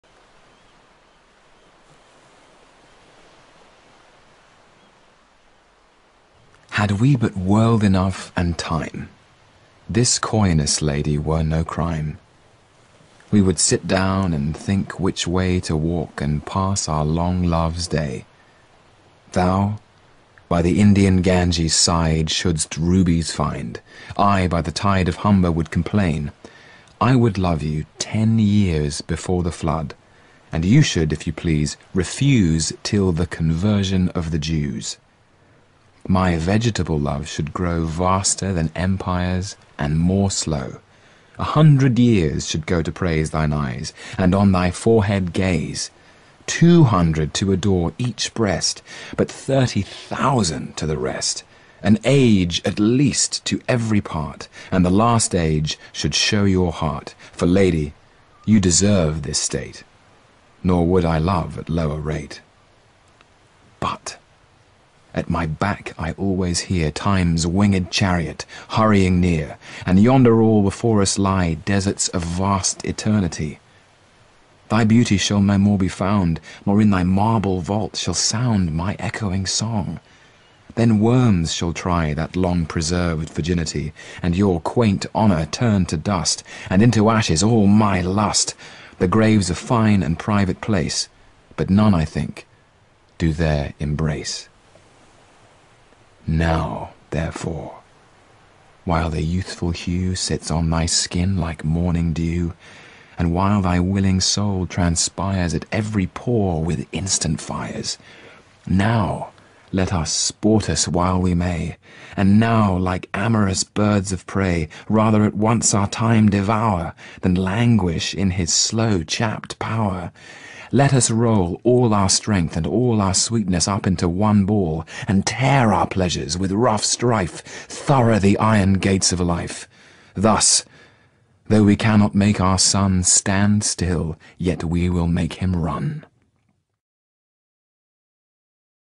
Tom Hiddleston Reading 'To His Coy Mistress' by ANDREW MARVELL
⭐ amzn.to/3NMJDDl

Poetry reading
Poetry ASMR